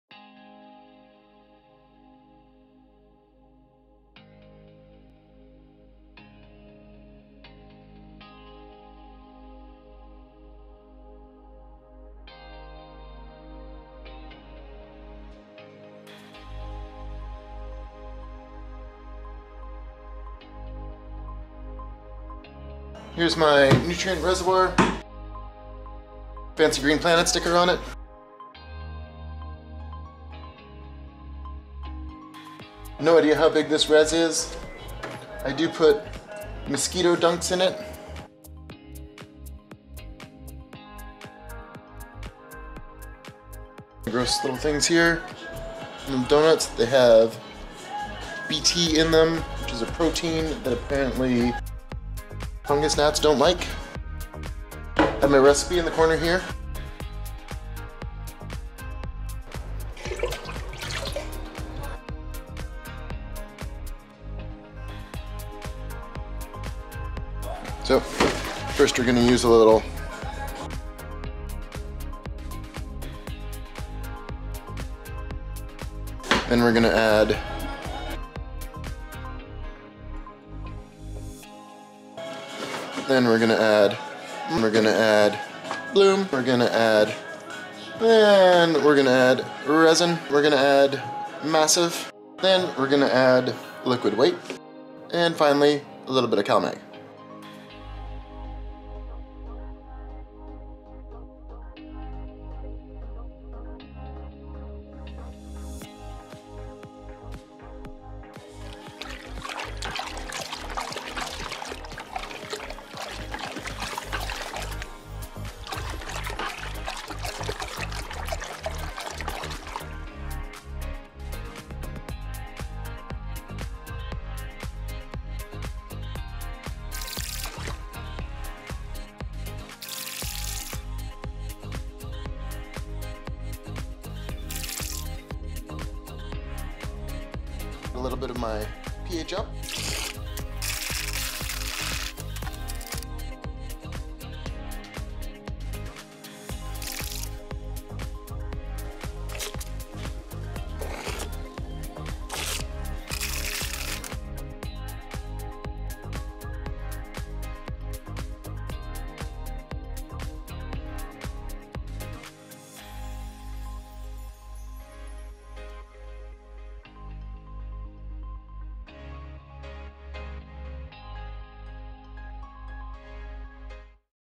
Mixing Green Planet nutrients for week three of flower
Adding the full line of Green Planet nutrients. Silica, Grow, Micro, Bloom, Rezin, Massive Bloom, Liquid Weight, Pro Cal.